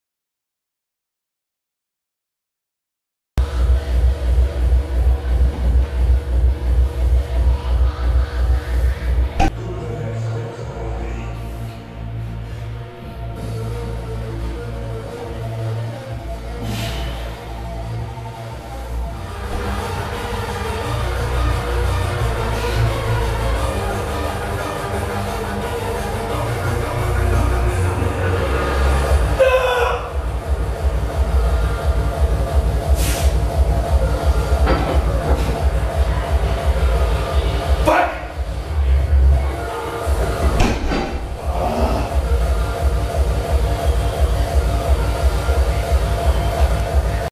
Deadlift
220, 240, 260 kg. Last one is a personal best.